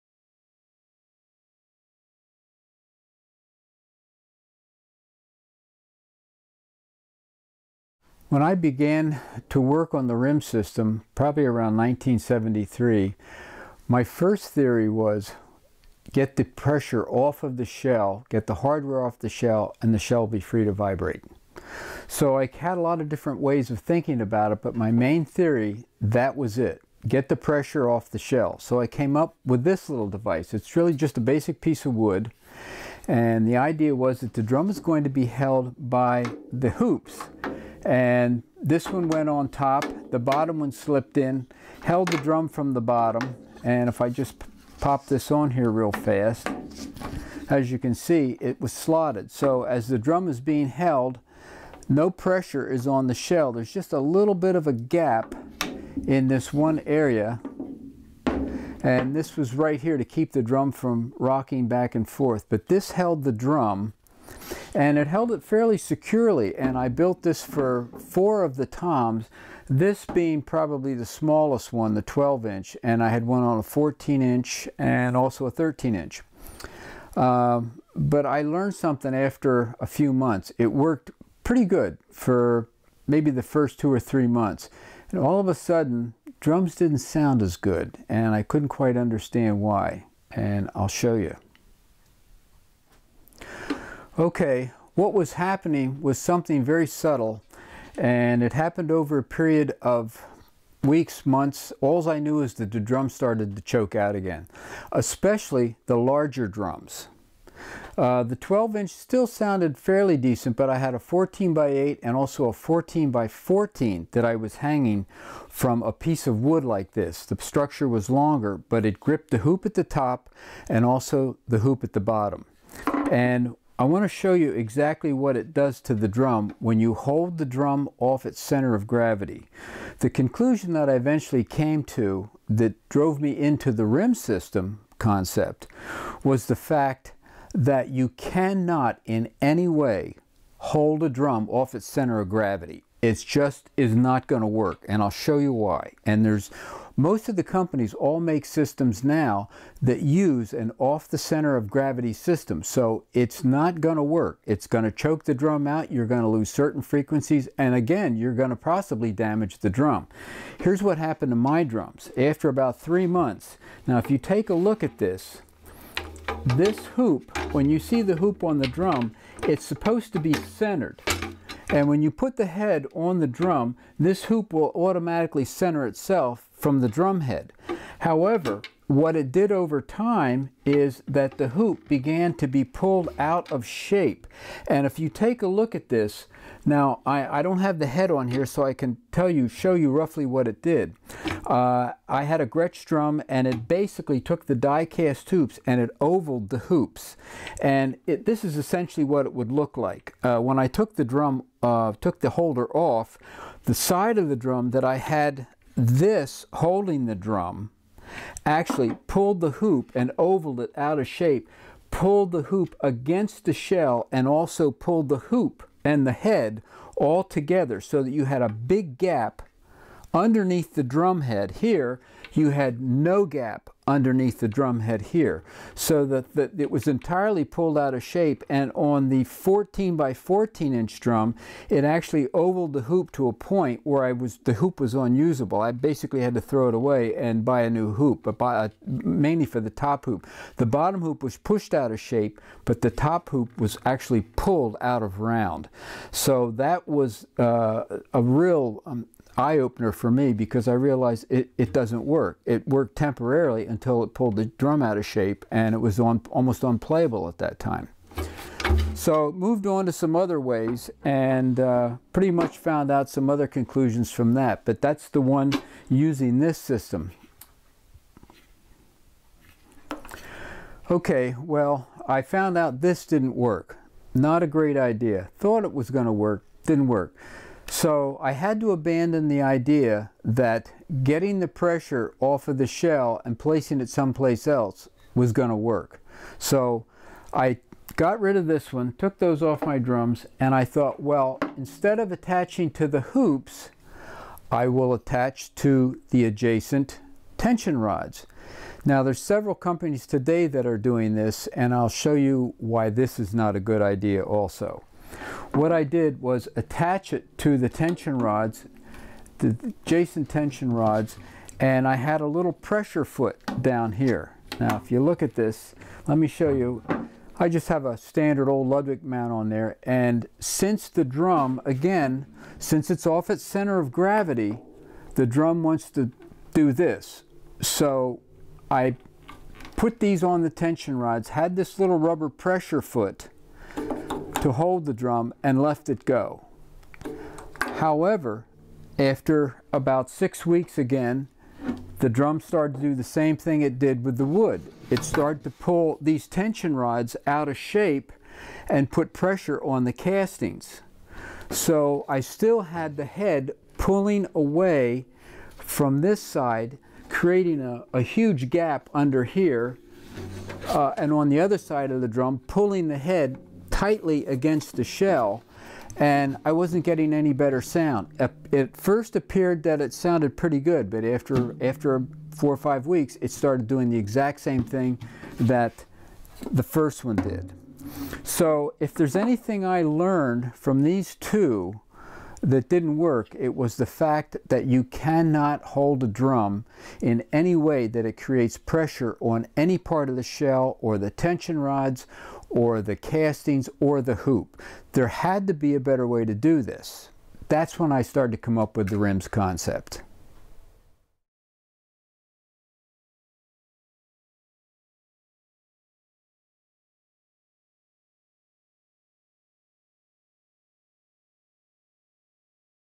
Early Attempts at Drum Suspension:Isolation
In this video, RIMS® inventor Gary Gauger describes and shows his initial attempts at drum suspension/isolation. He explores why his first few ideas did not work (each of which are now similar mounting hardware products from major drum companies) and how what he learned lead to the first RIMS® mount.